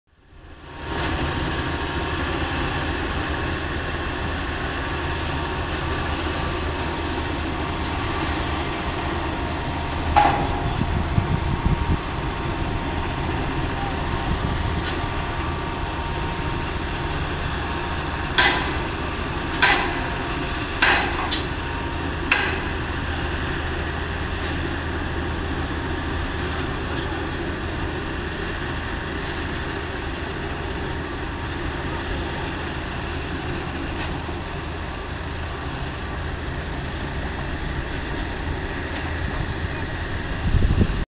3 cranes and 1 compactor working
One Roman diesel crane, 2 Ifa ADK cranes and one earth compactor working on a pipeline project